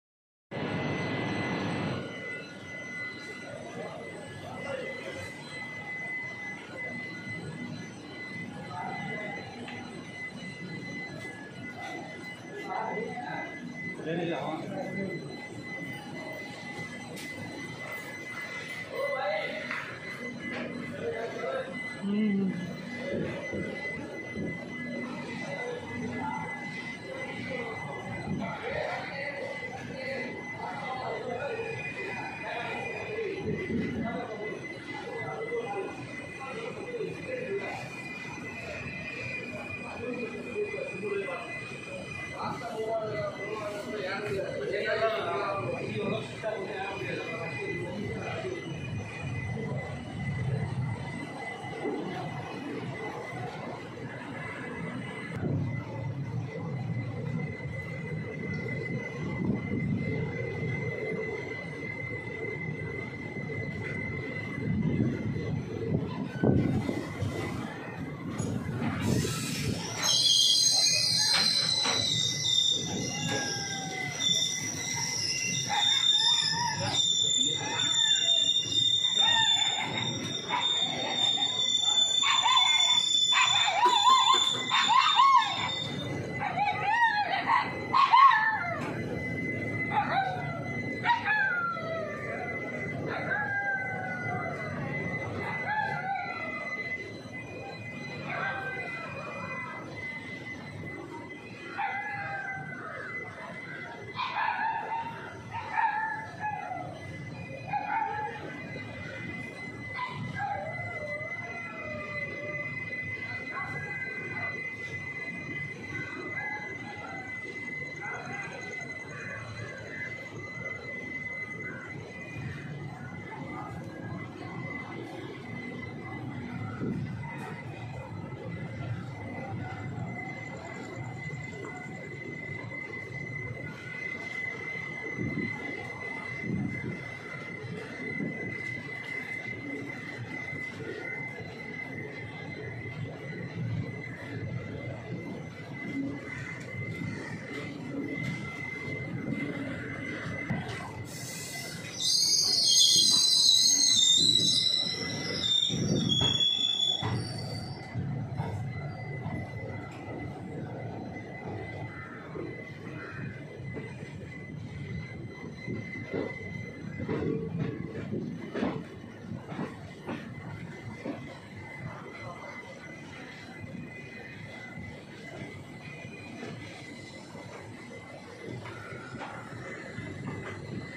11014 Coimbatore LTT covid special train departure from Coimbatore junction 25 April 2021
video taken at morning 8.55 am